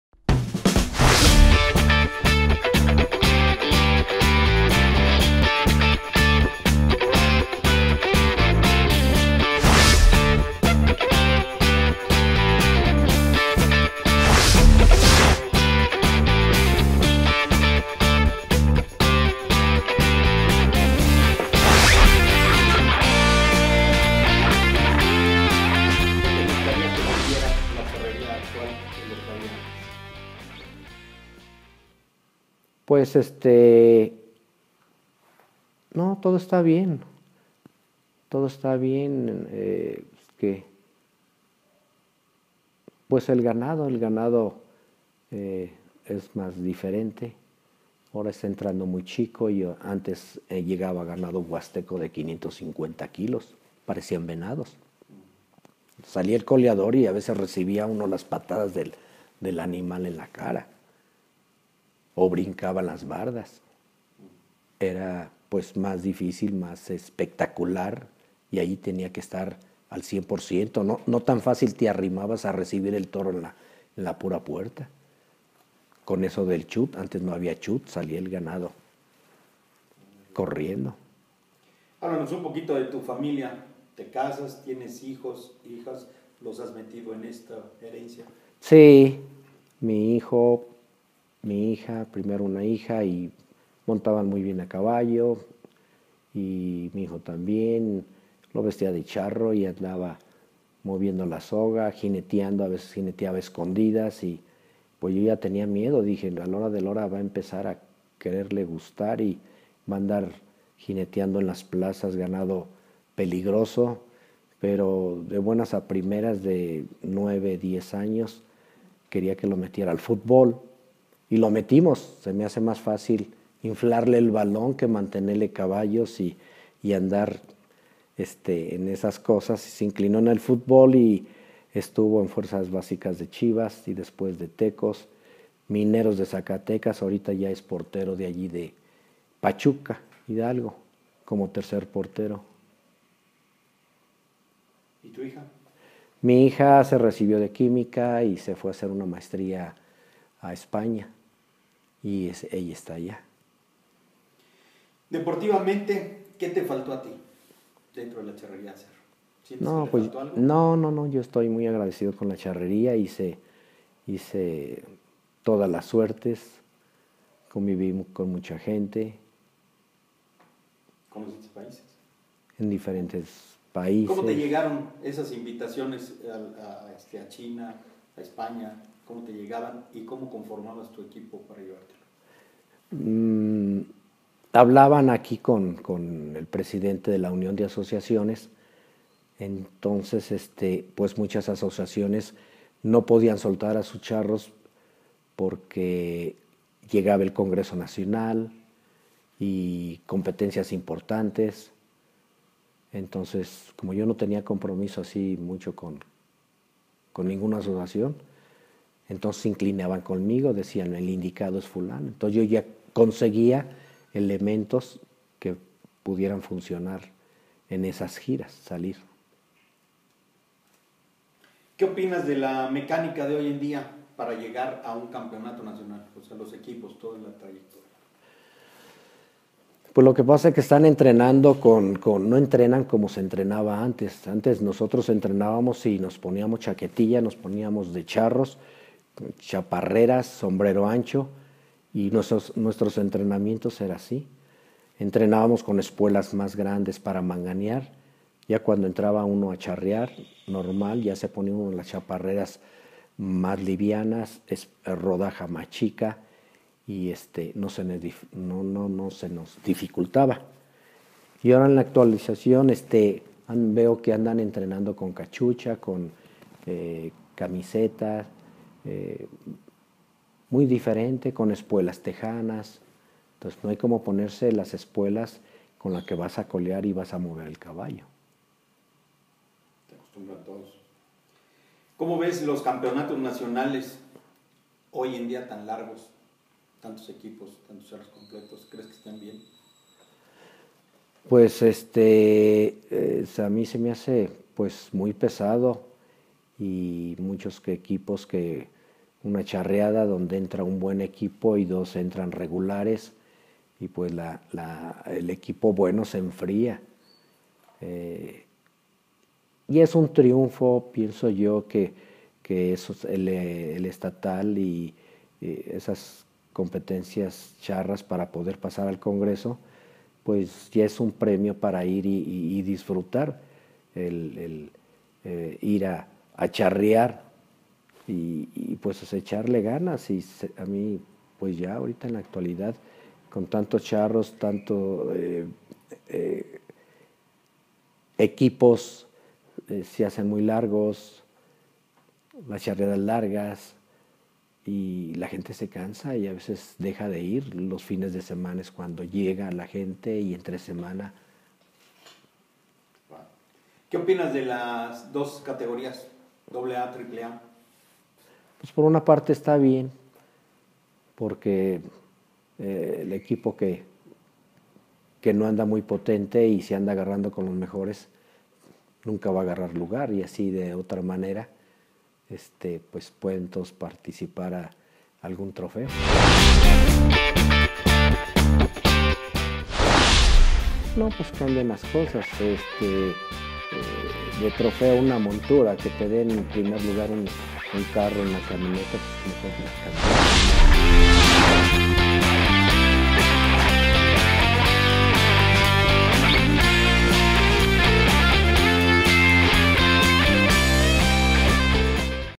Boletincharrocon |1ra Serie Capítulo 4| Rafael Ramírez "La Menudencia"
Los temas de hoy:
-Como era el ganado de colas antes y como es hoy día?
-A tus hijos los guiaste por este camino o tomaron otro destino?
-Te falto hacer algo dentro de la charrería?
-Como vez al deporte en la actualidad?
-Que opinion tiene de las categorias?